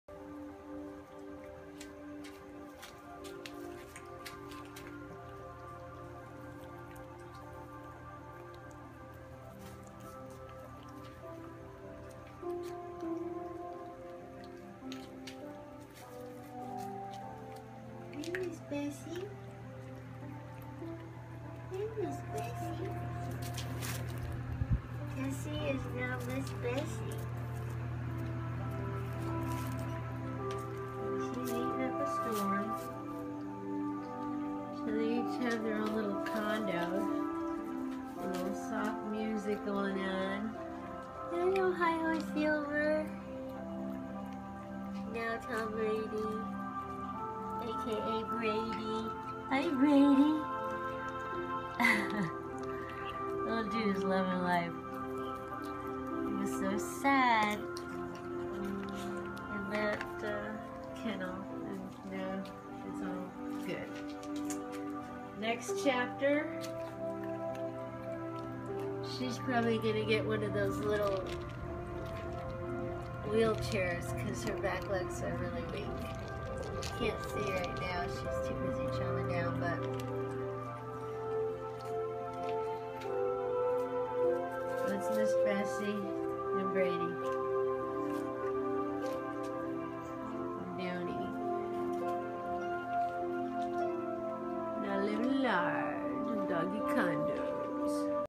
Miss Bessie and Tom Brady chillin in their doggie condos after being incarcerated at Downey Shelter.